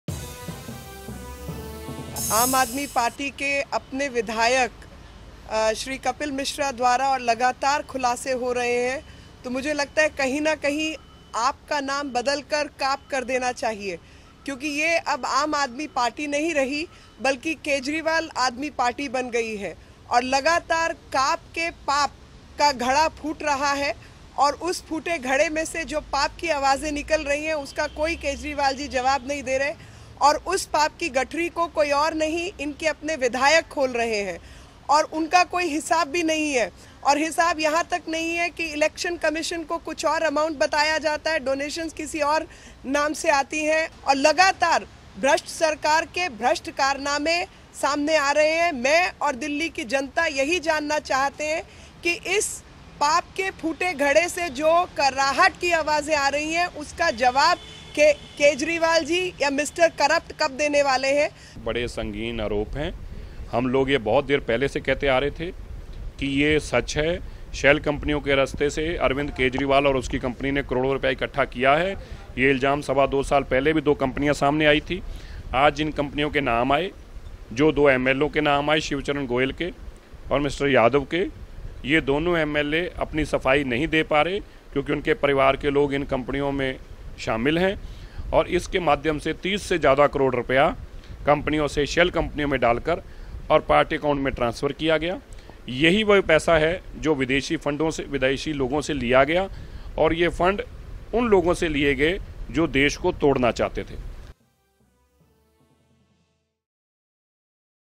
BJP ON AAP | बीजेपी ने कहा, 'आप' 'काप' है। 'आप' पर मढ़े भ्रष्टाचार के आरोप।
कपिल मिश्रा के आरोपो पर आखिरकार बीजेपी ने भी अपनी चुप्पी तोड़ी। पार्टी प्रवक्ता मीनाक्षी लेखी और मनजिंदर सिंह सिरसा ने कहा, कि आप भ्रष्ट है। उसने दिल्ली की जनता से ही नहीं, बल्कि उस आम आदमी को भी धोखा दिया जिसने उसको दिल्ली की कुर्सी पर बैठाया।